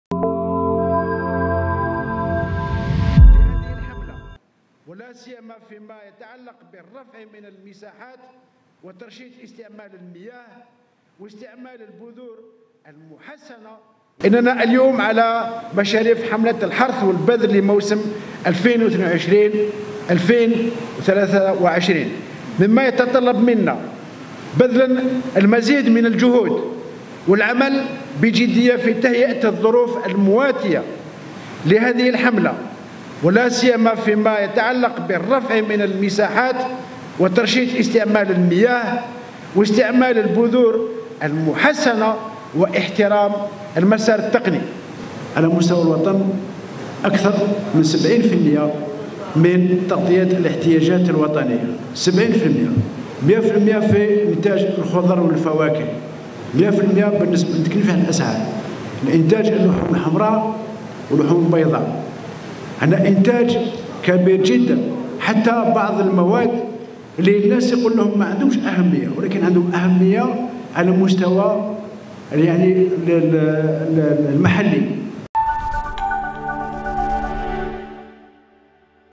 هني: تغطية أكثر من 70 % من الاحتياجات الوطنية من المنتجات الفلاحية
قسنطينة (وأج)- أكد وزير الفلاحة و التنمية الريفية  محمد عبد الحفيظ هني اليوم السبت بقسنطينة أن الجزائر قد استطاعت تلبية أزيد من 70 % من احتياجاتها من حيث المنتجات الفلاحية، موضحا في رده على انشغالات بعض ممثلي الفلاحين لولايات شرق البلاد أن النتائج المحققة في المجال الفلاحي تشير إلى تحقيق نسبة اكتفاء بلغت 100 %  بالنسبة لبعض الشعب على غرار إنتاج الخضر و الفواكه و كذا اللحوم الحمراء و البيضاء.